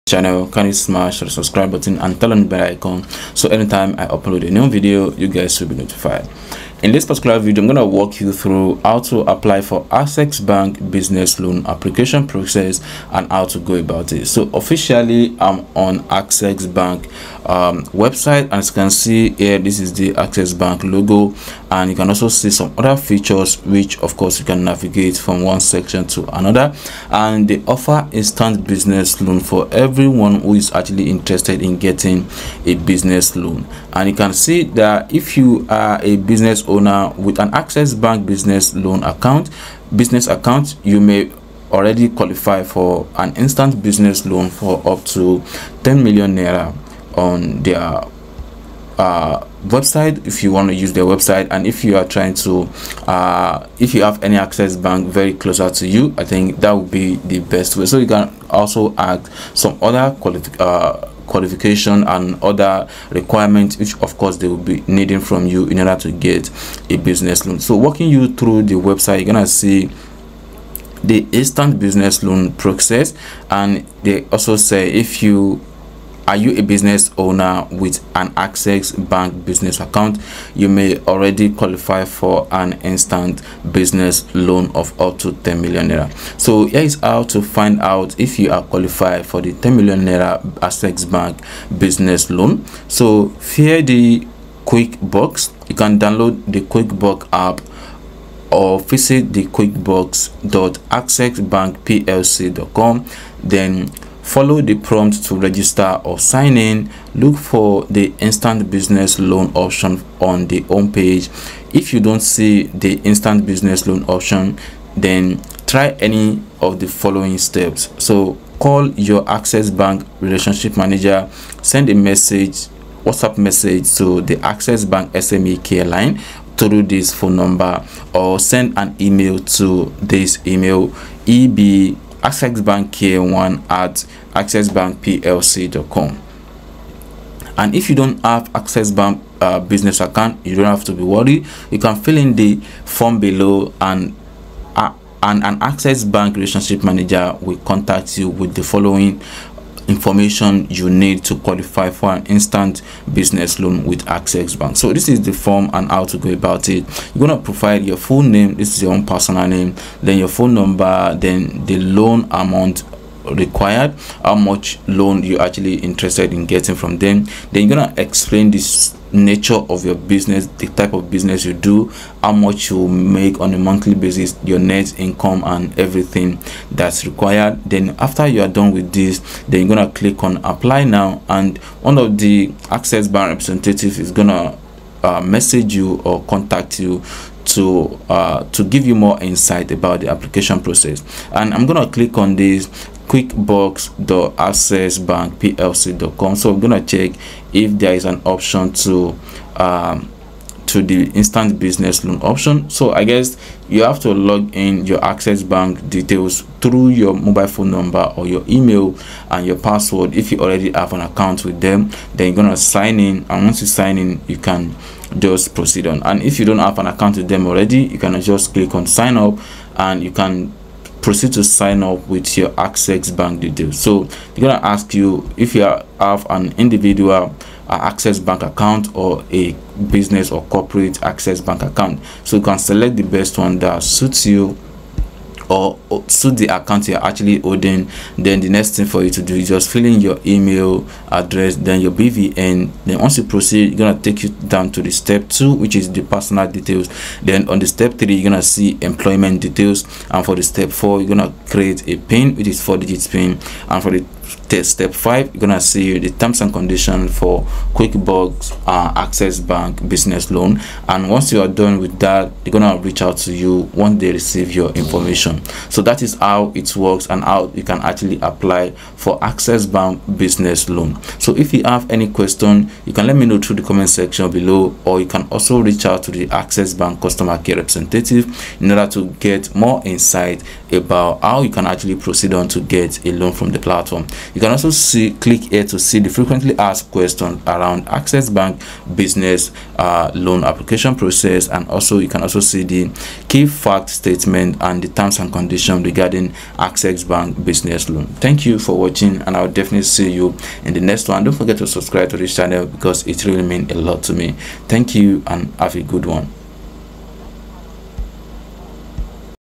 Access Bank Business Loan Application Process
In this video, i talk about  Access Bank Business Loan Application Process   . Don't forget to watch till the end because i shared more valuable information which will favor you.